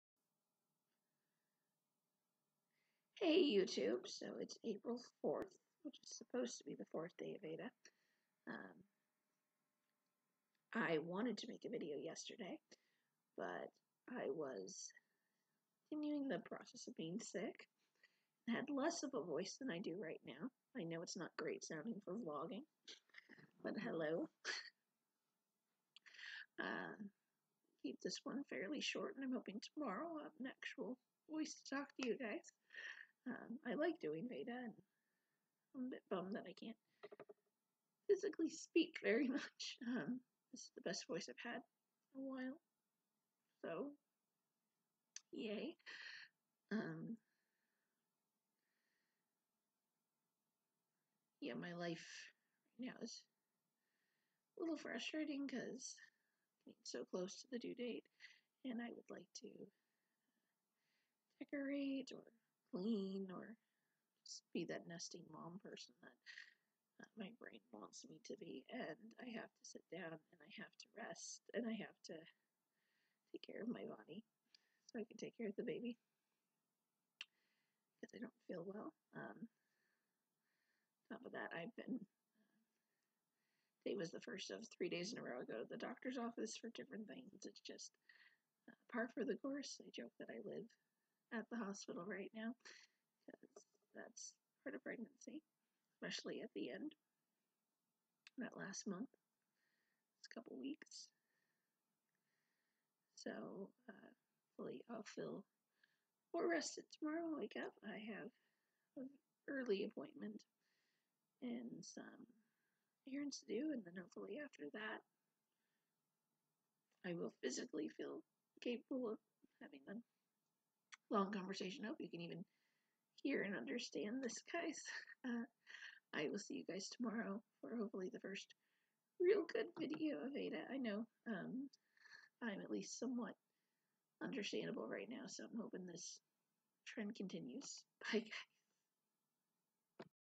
The Adventures of Sad Voice (VEDA 18 Day 4)
Another day of hospital visits and lack of voice.